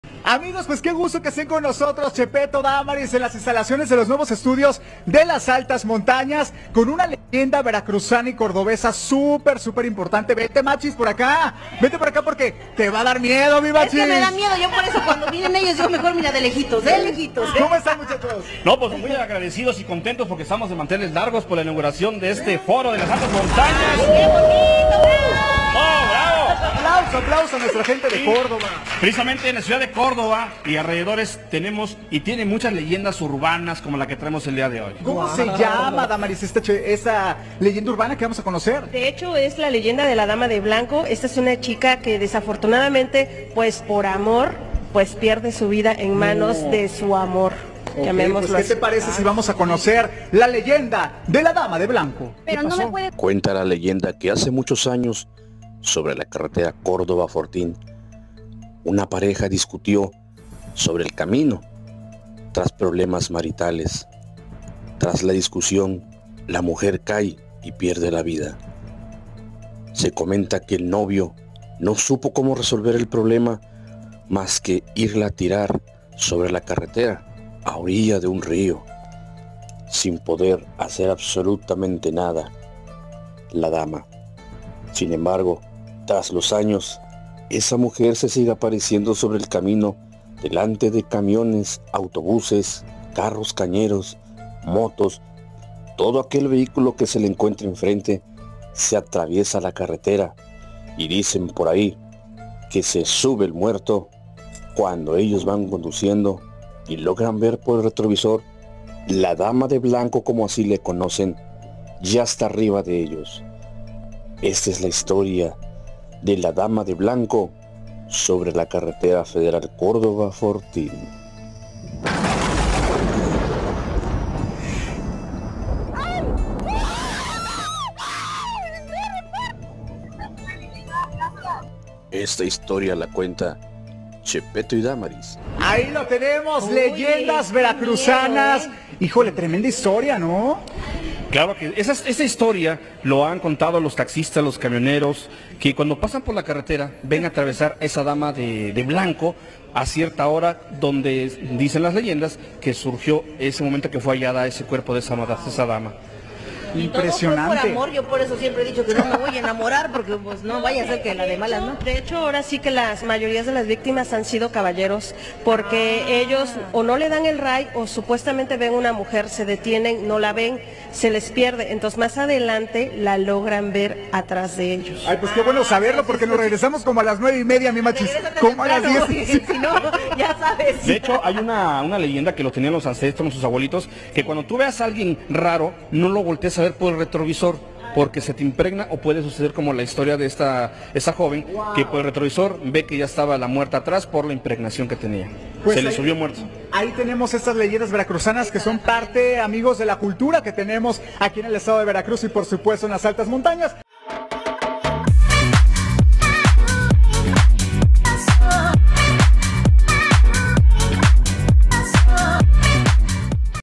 "La dama de blanco" es una leyenda muy popular en las Altas Montañas.
leyendasveracruzanas | "La dama de blanco" es una leyenda muy popular en las Altas Montañas.